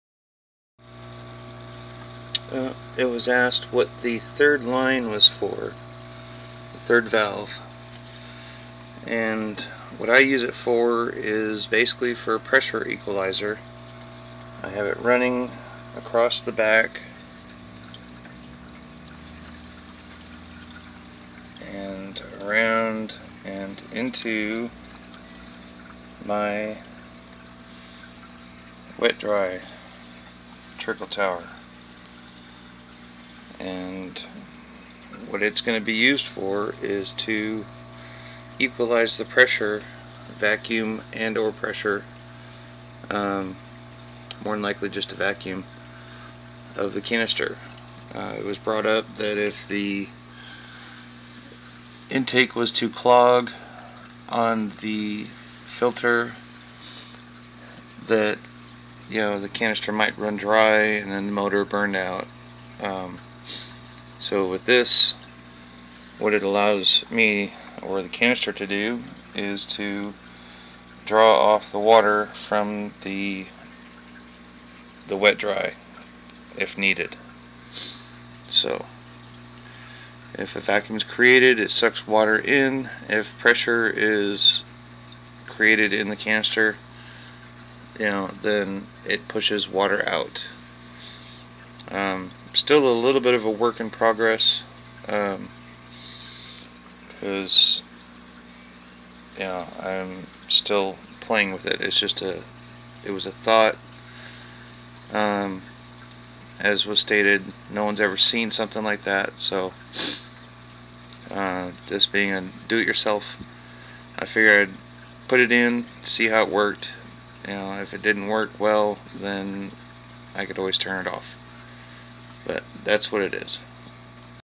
Third Valve Description
I had installed a 3rd Valve on my DIY Canister filter. This is my reasoning behind it.